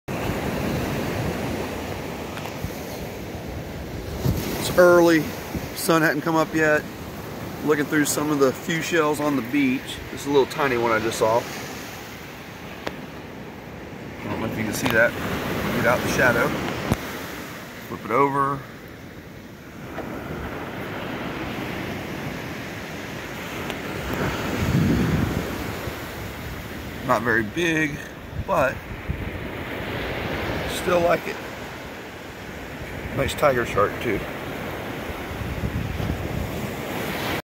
Shark teeth hunting before daybreak and found a colorful tiger shark tooth. Enjoy!
Love to get out before the day walkers.  Was able to find a colorful tiger shark tooth this morning at the local beach.  These are some awesome shark's teeth that can be found here fairly often.  Enjoy!